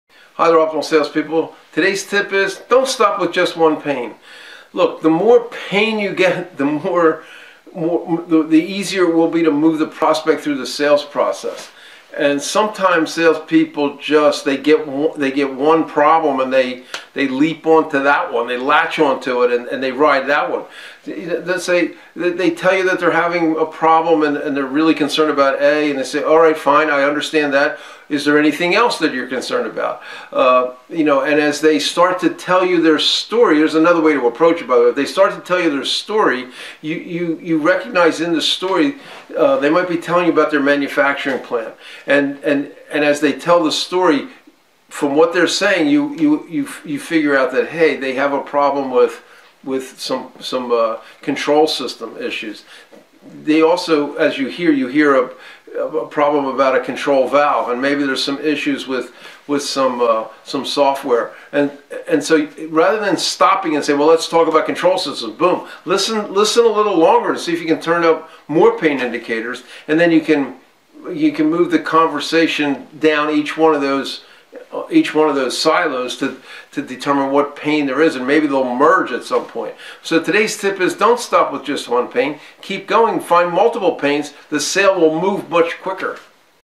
Don't Stop with just one pain
The more pain you have the bigger the compelling reason to move forward with the project. So, don’t stop with the first pain you hear. After you get some of the details ask if there is anything else. You might ask how this will affect another part of the facility which might expand the scope (and price) of the project. Don’t get so excited about this project that you forget everything else. Take a step back and take a broader view. What else is this person working? Is this problem an indication of other and possibly bigger issues? In what context does this problem exist? The answer to that question could lead you to other things. The message here is brief but powerful. Don’t stop at the first problem you run into.